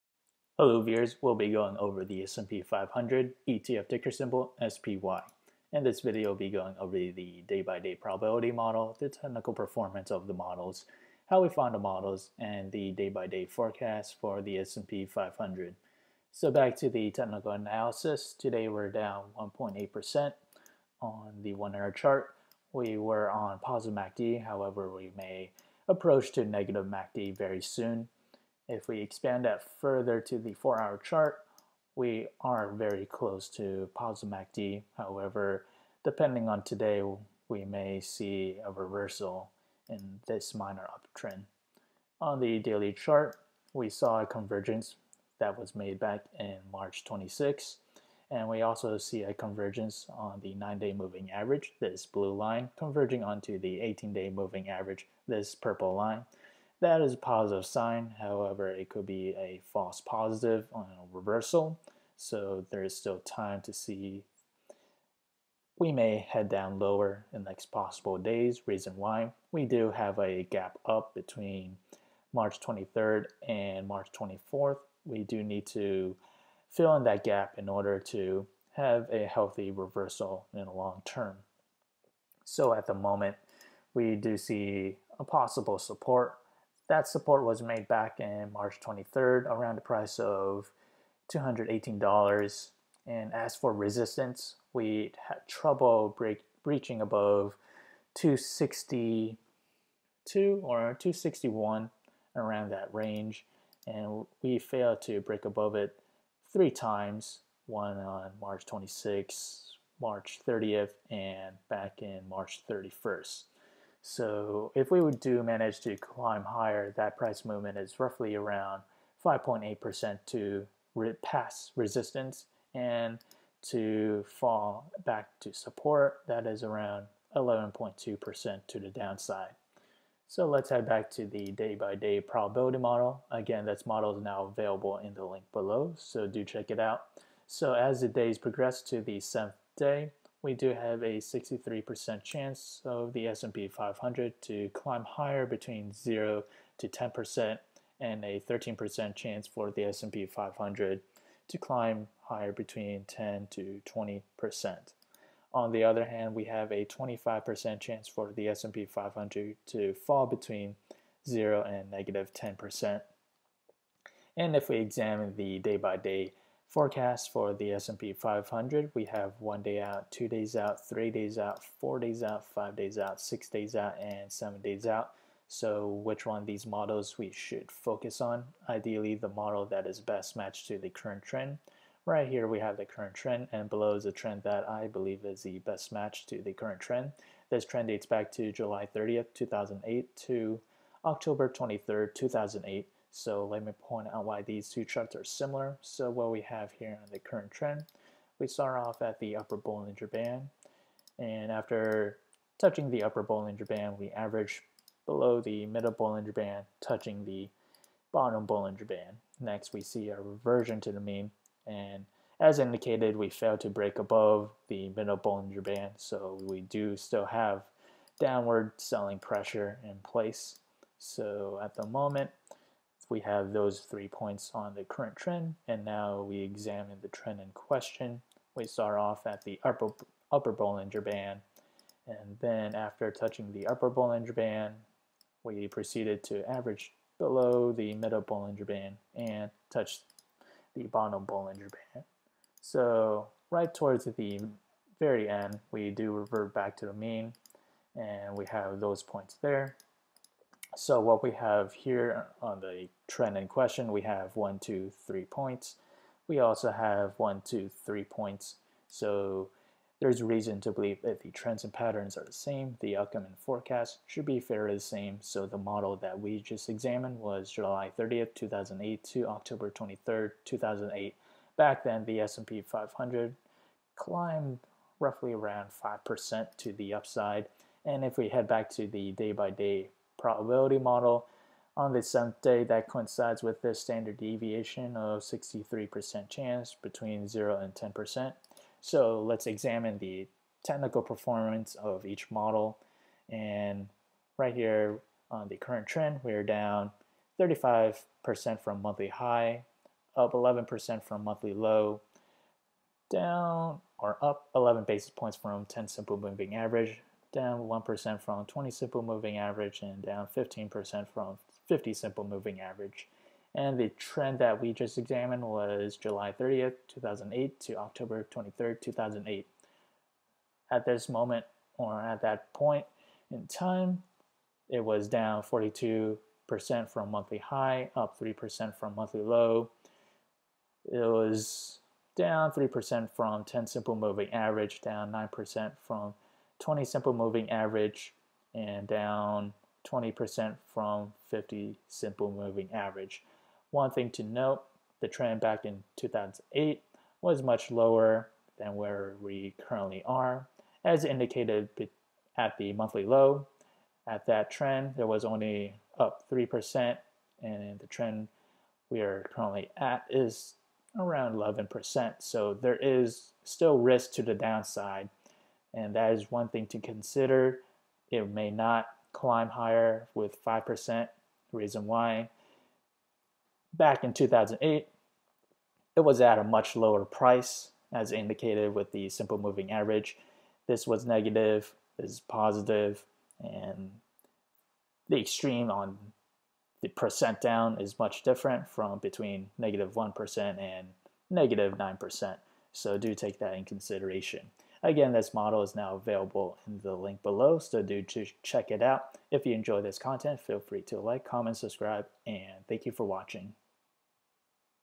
S&P 500 [SPY] April 3 2020 Forecast / Probability
SPDR S&P 500 Trust ETF (SPY) is an ETF or exchange traded fund. It is designed to track the S&P 500 index. SPDR stands for Standard & Poor's Depositary Receipts. The SPY is the largest ETF in the world as more and more investors seek cheaper and passive investments. Other ETFs that track the S&P 500 are Vanguard S&P 500 ETF (VOO) and iShares S&P 500 Index (IVV).